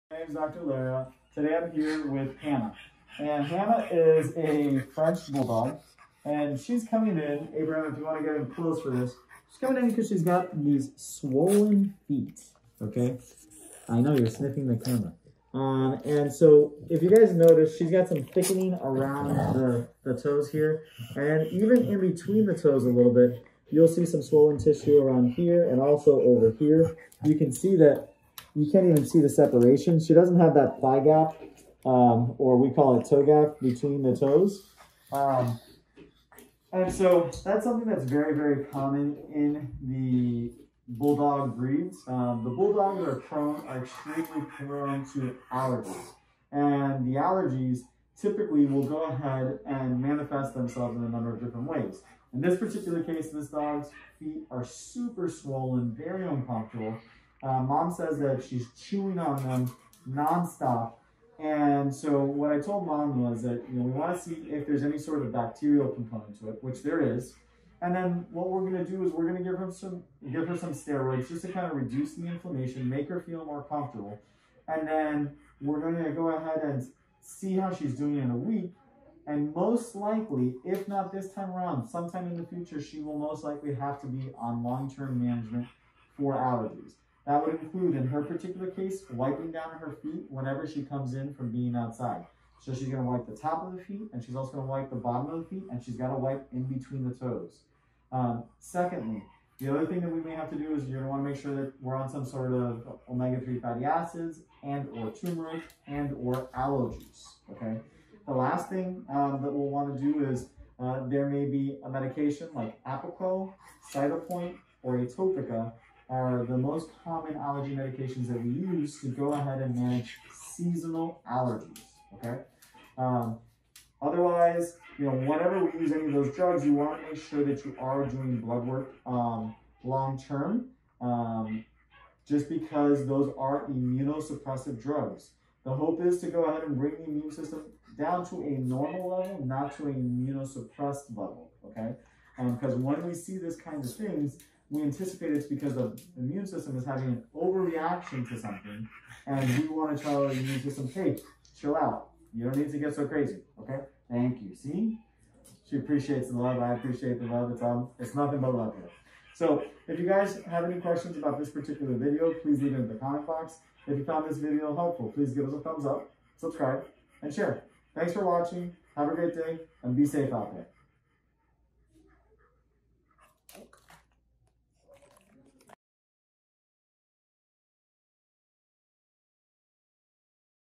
Itchy Paws from Seasonal Allergies | What to Do
Dr. Lera talks about itchy, red & swollen paws (pododermatitis) caused by seasonal allergies with Hana, a four-year-old French Bulldog.

Seasonal allergies work in dogs a lot like they do in humans. When a dog is exposed to an allergen for the first time, there will be no visible response to the allergen, but their immune system goes to work producing antibodies to the allergen. The second time your dog is exposed to the allergen, those antibodies that were created during the first exposure go to work fighting off the allergen and cause an allergic reaction. Each time your pet is exposed to the allergen, their immune system fights harder and harder against the allergen causing a bigger allergic reaction each exposure.